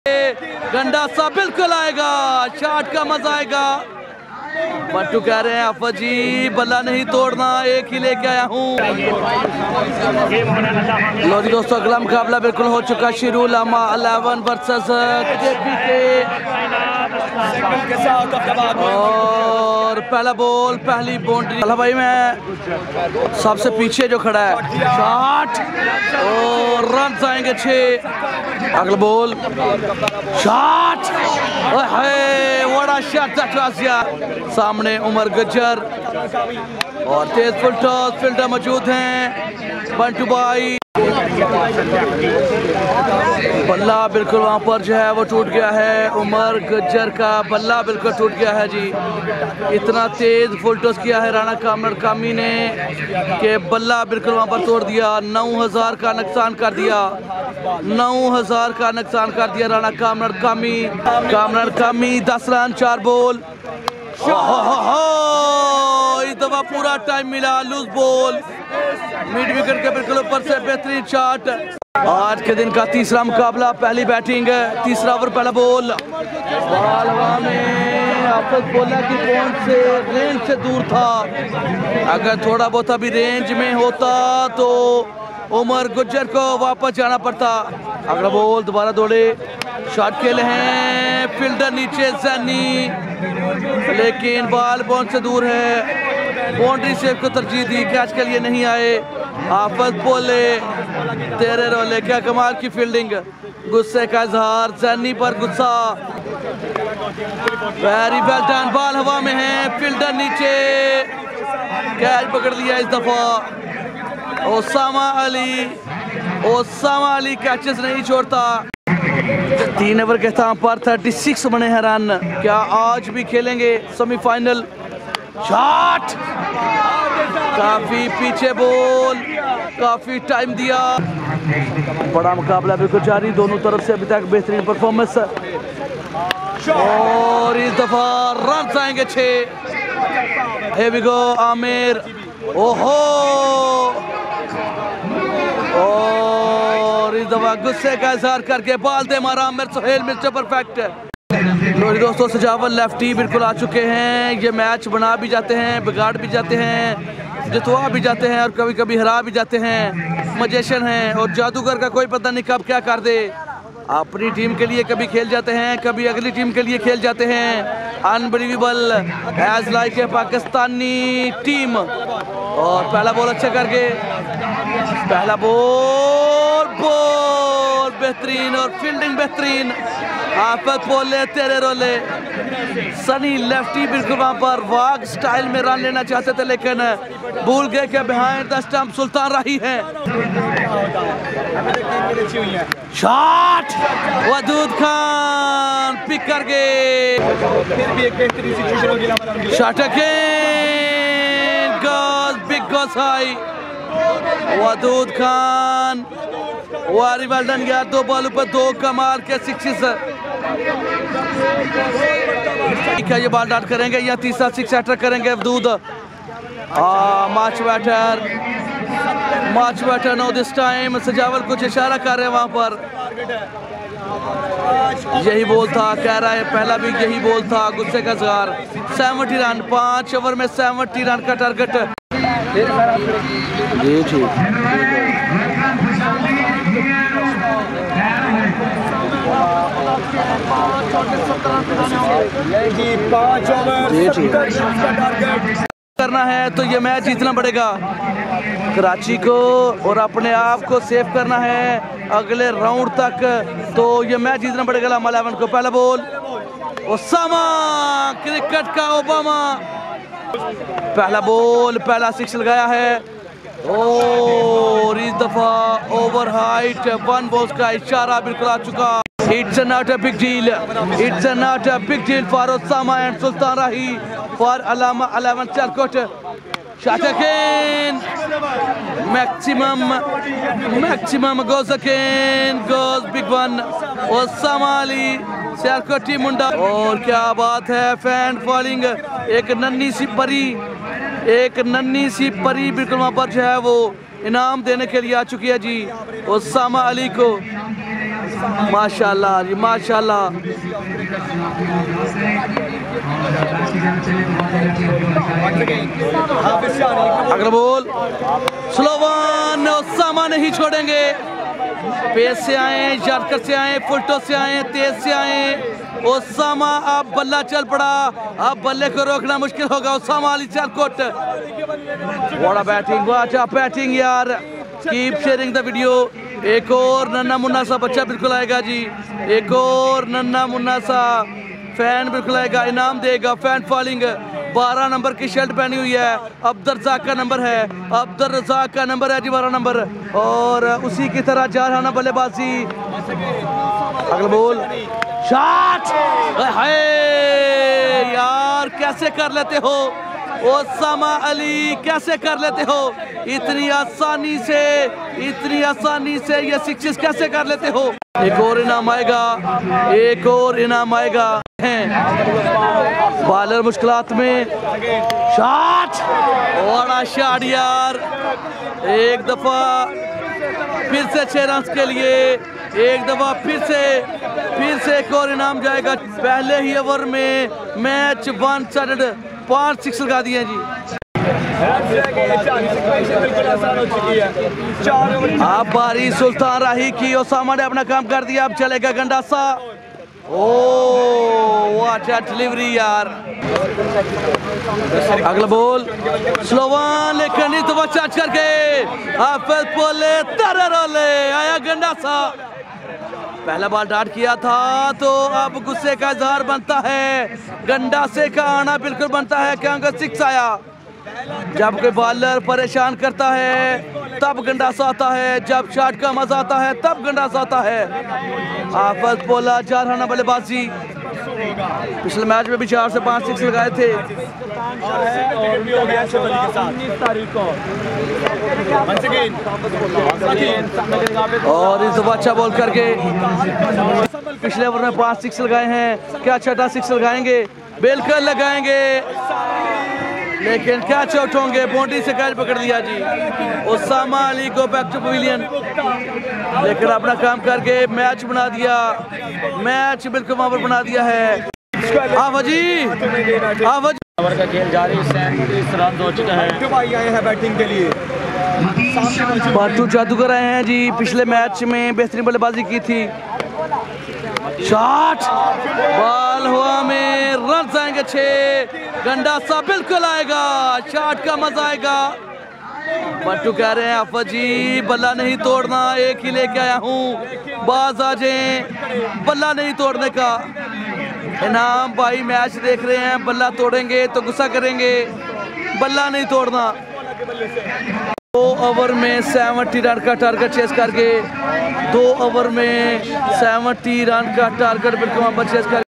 12 BALL CHASE 70 RUNS AMAZING BATTING BY USAMA ALI SIALKOTI
HAFAZ POLLA VS UMAR GUJJAR USAMA ALI VS WAJID LEFTY GERAT MATCH